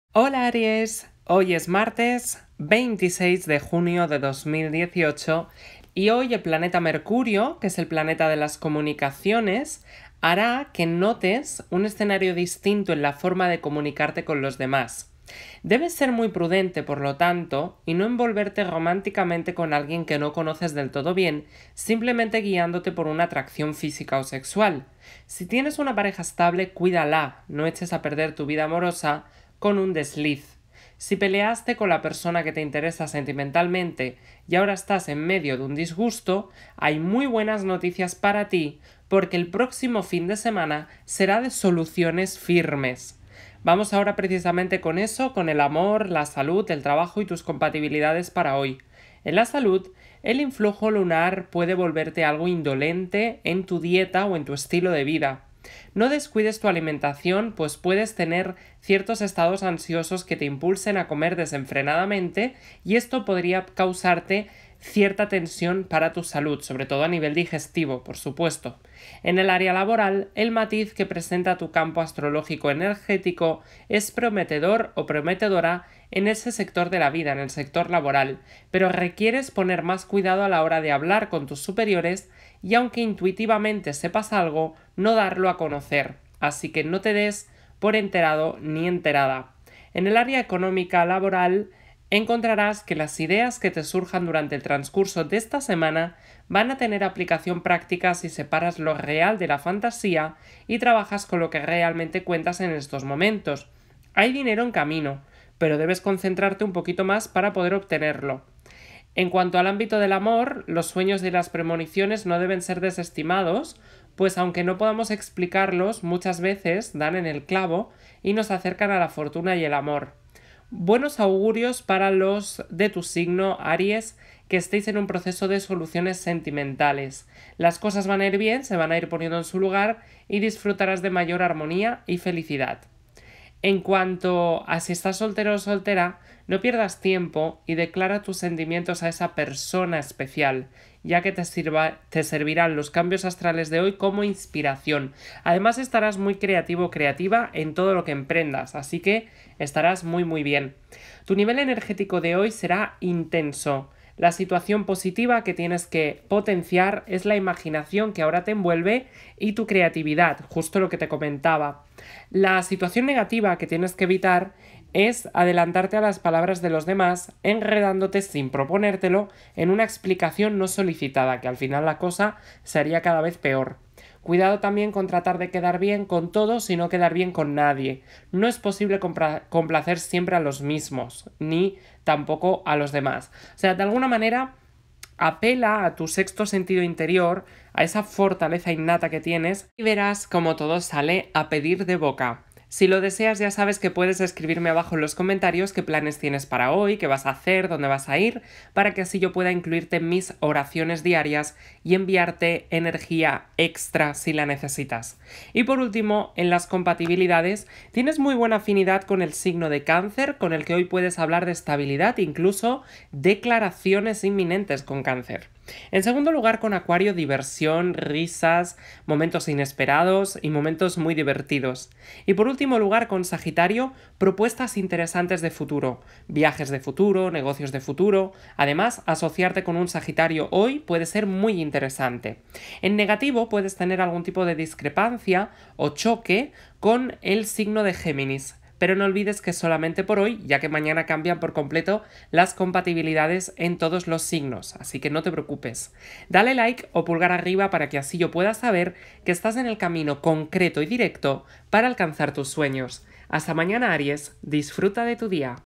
Horóscopo Diario - Aries - 26 de Junio de 2018
Horóscopo de hoy Aries 26 de Junio de 2018. Horóscopo gratis.
Descubre tu futuro con mi horóscopo diario para ti, mi querido signo Aries en el amor, el trabajo, la salud, el dinero y las compatibilidades con otros signos para hoy.